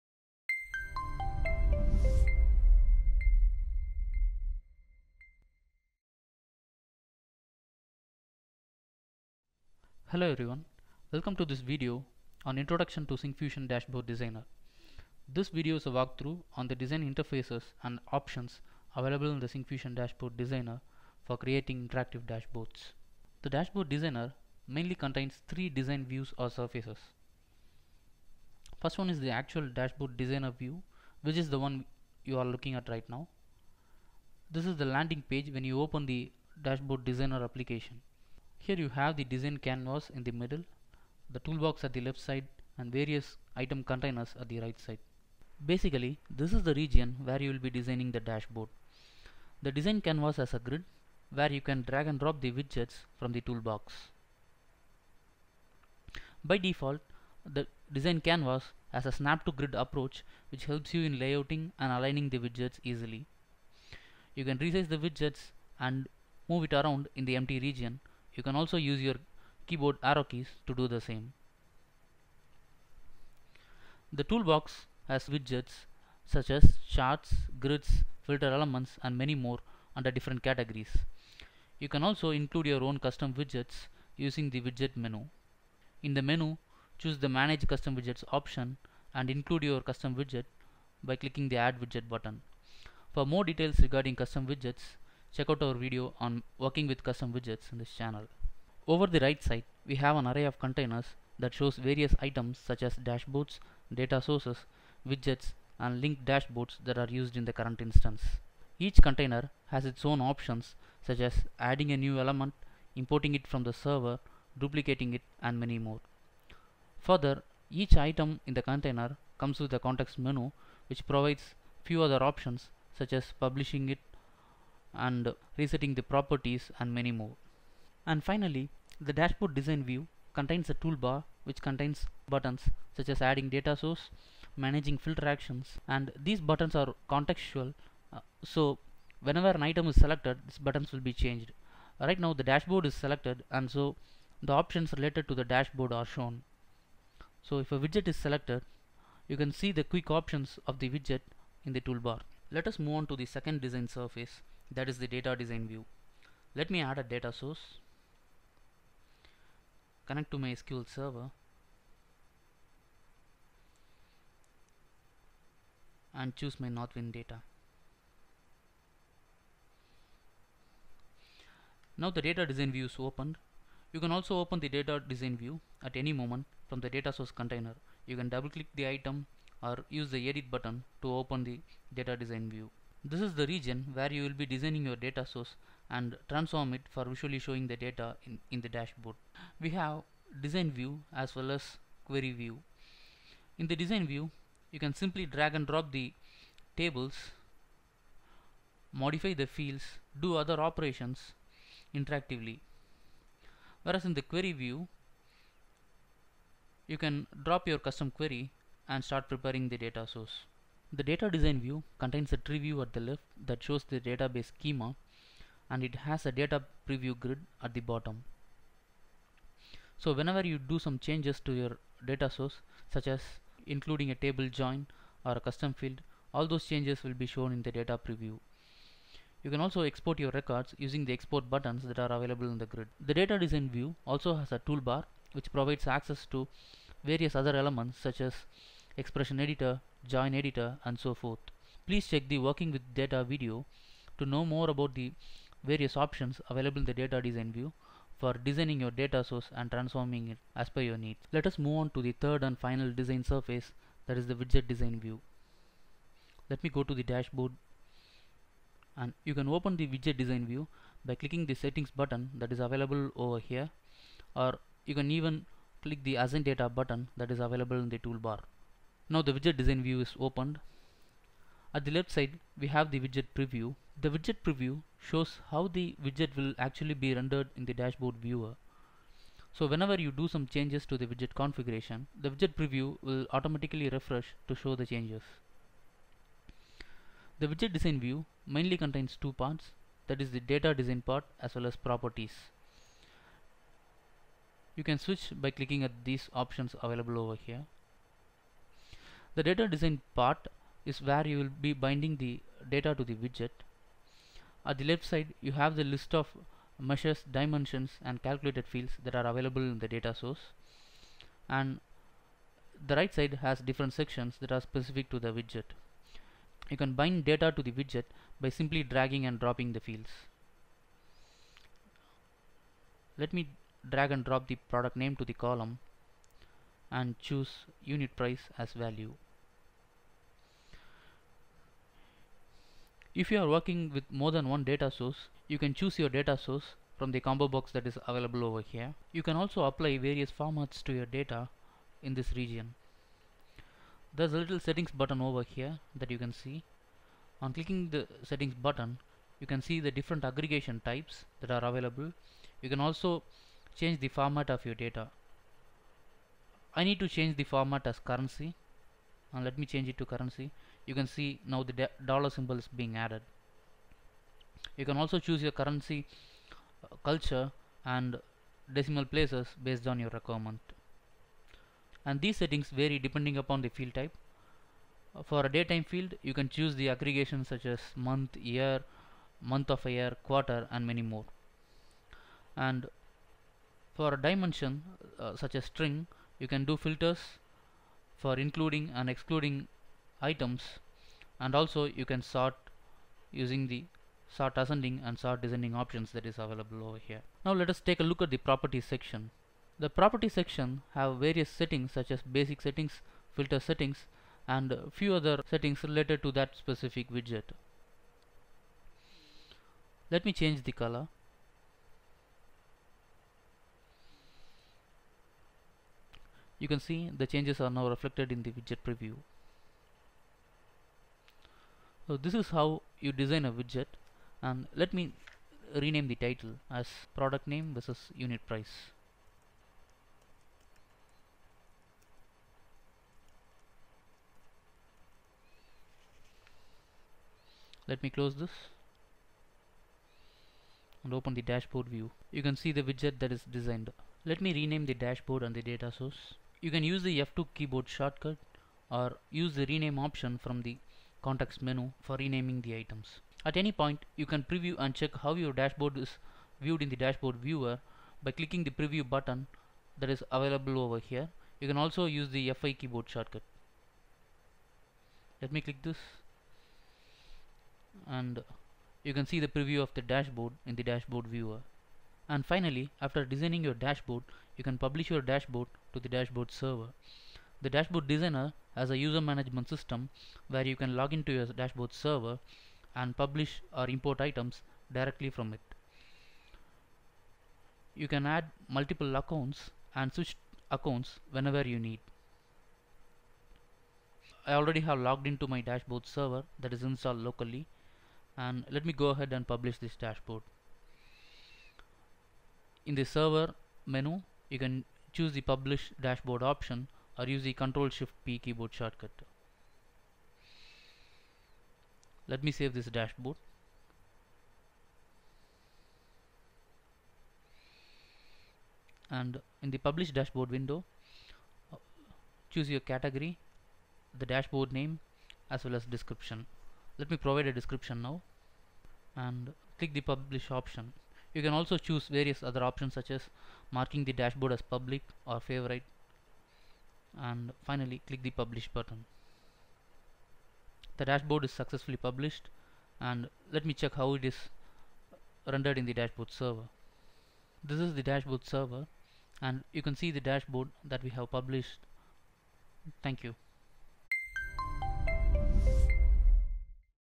The Dashboard Designer Interface
The Syncfusion Dashboard Designer application lets you connect to data and visualize it as dashboards using data visualization and filter widgets. This video explains the various design interfaces available in the application for creating rich business dashboards.

Documentation Links